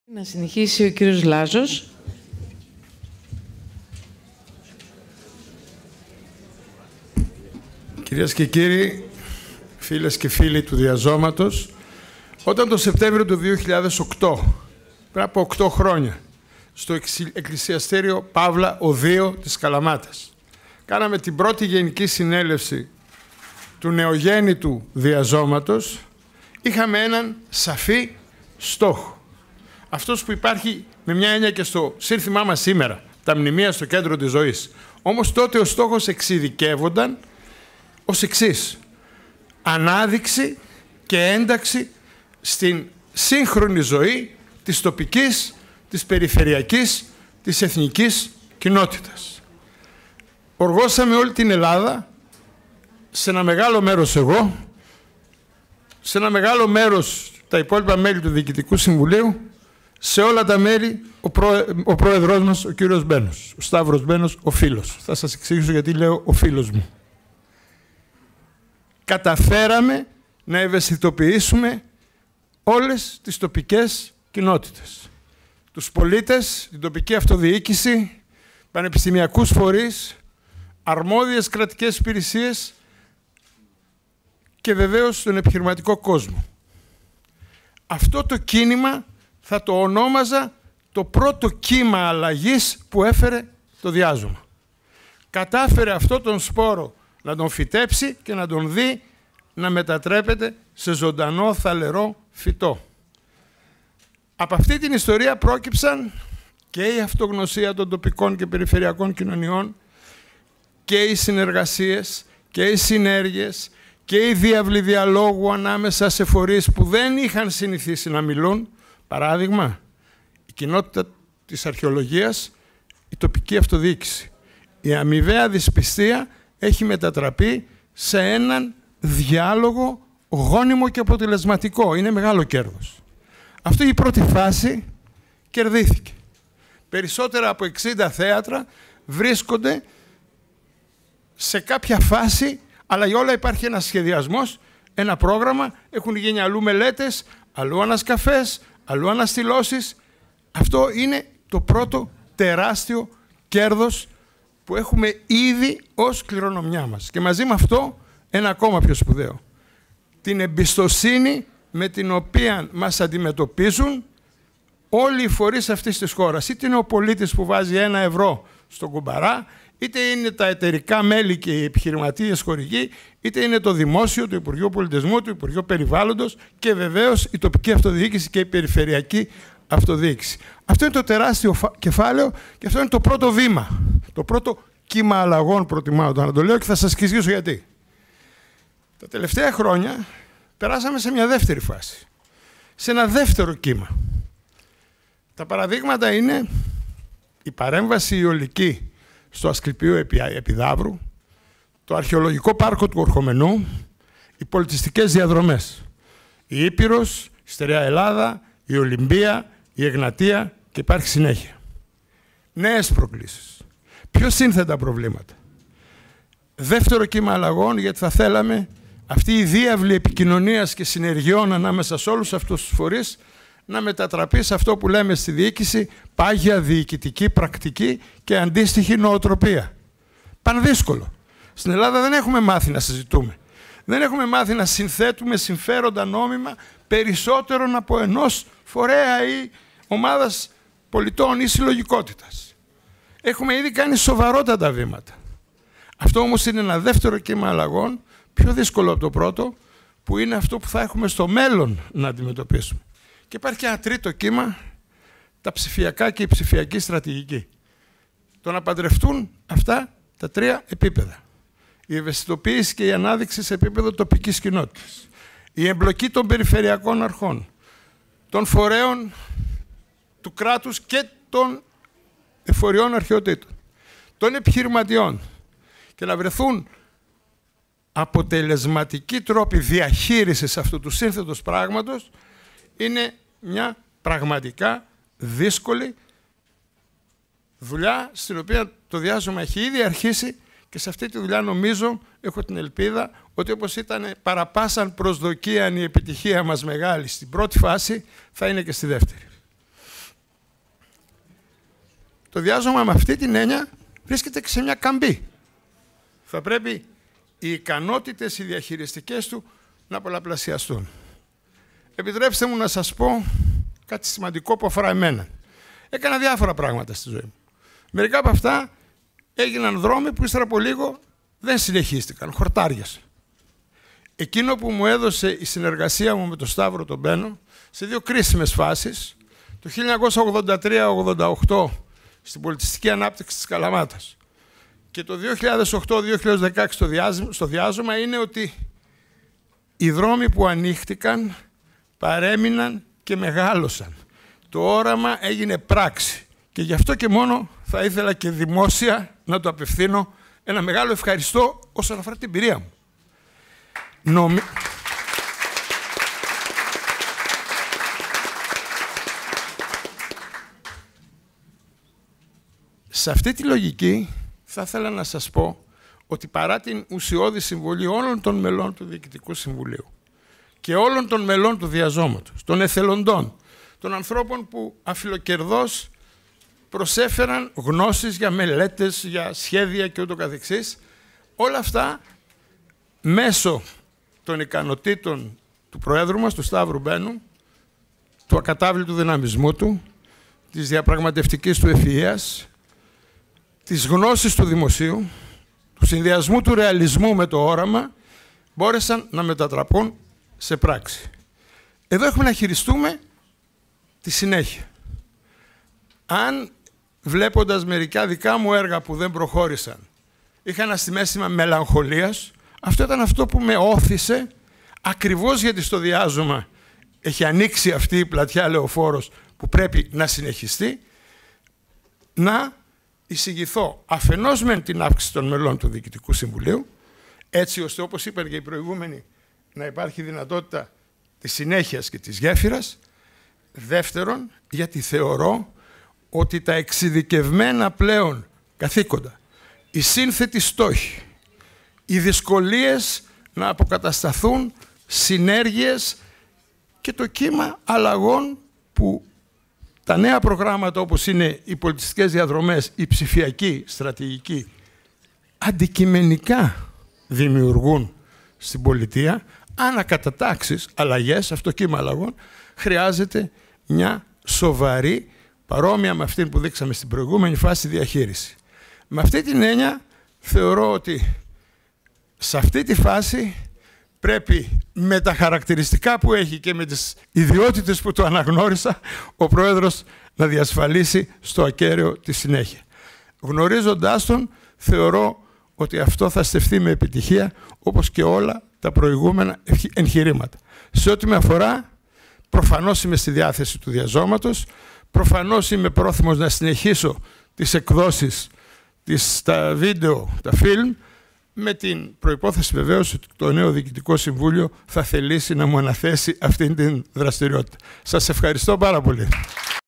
Απολογισμός Δράσεων Σωματείου - Χρήστος Λάζος
Ο κύκλος των εργασιών της Συνέλευσης ολοκληρώθηκε την Κυριακή 18 Σεπτεμβρίου στο ξενοδοχείο "AMALIA" του Ναυπλίου. Εκεί πραγματοποιήθηκε ο απολογισμός της δράσης του απερχόμενου διοικητικού συμβουλίου και ο προγραμματισμός των δράσεων για το επόμενο έτος. Κύριο θέμα του απολογισμού ήταν η παρουσίαση νέων προγραμμάτων και δράσεων, όπως οι Πολιτιστικές Διαδρομές στα αρχαία θέατρα και τα Αρχαιολογικά Πάρκα, προκειμένου τα μνημεία, αφού συντηρηθούν, να τεθούν στο κέντρο της πραγματικής ζωής, της αειφορίας και της βιώσιμης ανάπτυξης .